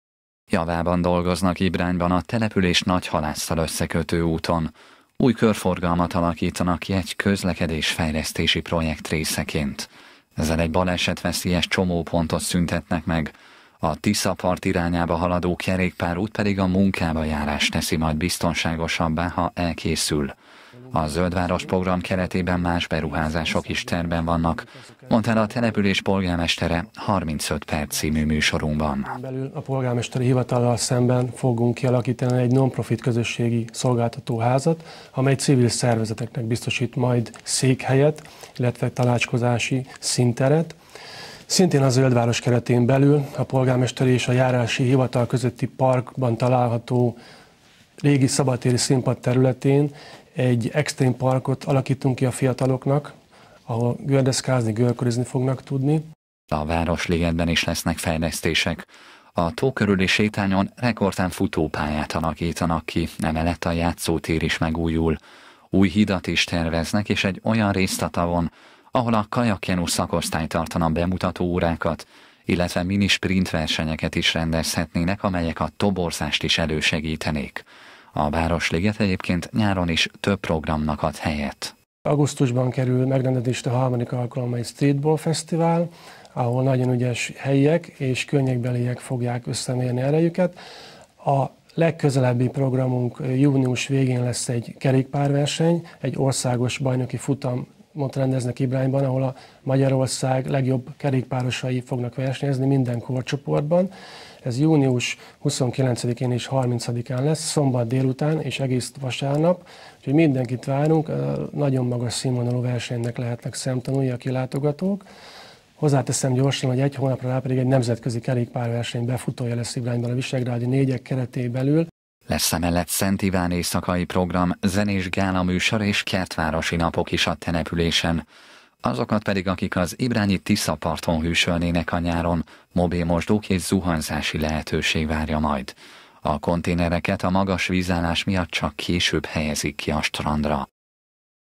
Fejlesztések Ibrányban - Kölcsey Tv
Új körforgalom és kerékpárút, extrém sportpark, mobil mosdók a Tisza-parton - számos fejlesztés valósul meg Ibrányban. A “Zöld város” projekt részeként pedig a Városligetben is lesznek újdonságok. A település nyáron több programmal - mások mellett streetball bajnoksággal - várja a helyi lakosokat és máshonnan érkezőket.